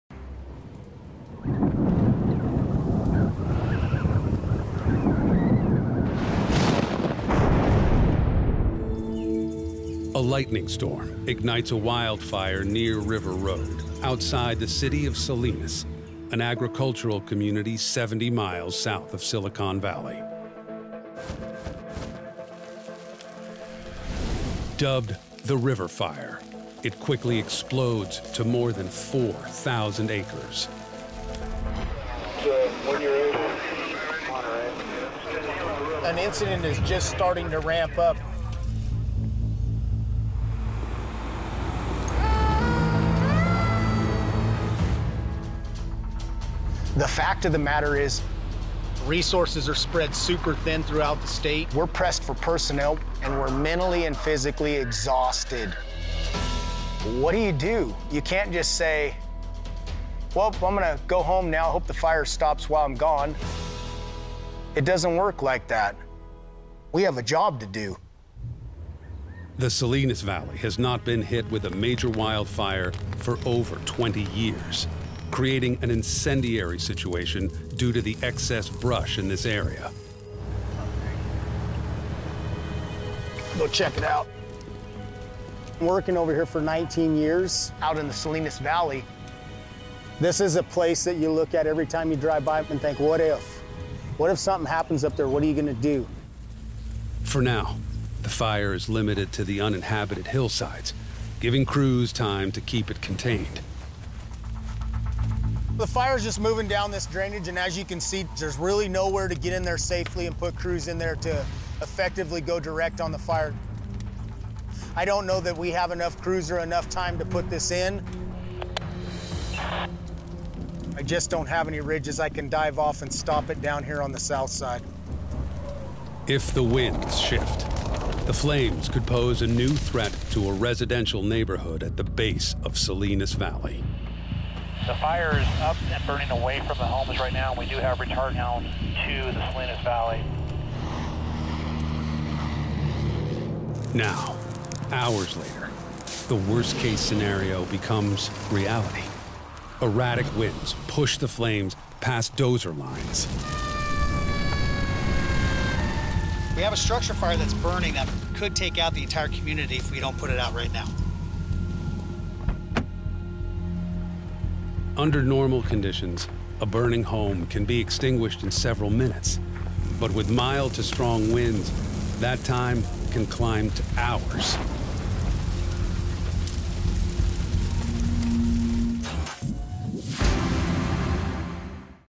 The River Fire | Cal Fire
The Salinas valley in California has not been hit with a major wildfire for over 20 years. All the excess brush in the area has created an incendiary situation perfect for the blaze to thrive. The fighters are prepared... but the worse case scenario is about to become reality.